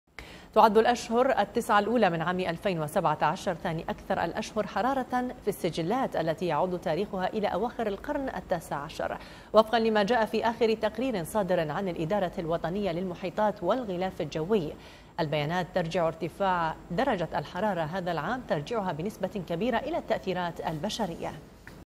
أخبار منوعة | 2017 ثاني اكثر درجات الحرارة ارتفاعا منذ القرن التاسع عشر
تعد الاشهر التسعة الاولى من عام 2017 ثاني اكثر الاشهر حرارة في السجلات التى يعود تاريخها الى اواخر القرن التاسع عشر، وفقا لما جاء فى اخر تقرير صادر عن الادارة الوطنية للمحيطات والغلاف الجوي ..
البيانات ترجع ارتفاع درجة الحرارة هذا العام بنسبة كبيرة الى التأثيرات البشرية.

 الفترة من يناير إلى سبتمبر سجلت 0.87 درجة مئوية (1.57 درجة فهرنهايت) وهي أعلى من متوسط القرن العشرين.

تسع فترات من اصل أحر عشر فترات تقع من يناير حتى سبتمبر  بالنسبة لدرجة حرارة الارض والمحيطات وقعت خلال القرن ال 21 (منذ عام 2005)، مع سنة واحدة فقط من القرن 20 وهي سنة 1998 من بين  أعلى 10.

ورجحت الادارة الوطنية للمحيطات والغلاف الجوي  ان تكون سنة 2017من بين اعلى ثلاث سنوات حرارية مسجلة

وكانت الأعوام الثلاث الماضية سجلت ارتفاعاً في الحرارة، مقارنة بتسعينات القرن الماضي

وبغض النظر عن الاختلاف في المواقف السياسية من القضية البيئية، إلا أن 30% من الأجناس الحيوانية تواجه خطر الانقارض نتيجة درجات الحرارة المترفعة عاما تلو الآخر، وفق ما أعنلت منظمة " IPCC" التابعة لمنظمة الامم المتحدة وبرامج البيئة التابعة لها.
كما حذرت من أن عدم اتخاذ خطوات لتفادي تداعيات الاحتباس الحراري، فإن الثمن سيكون باهظاً.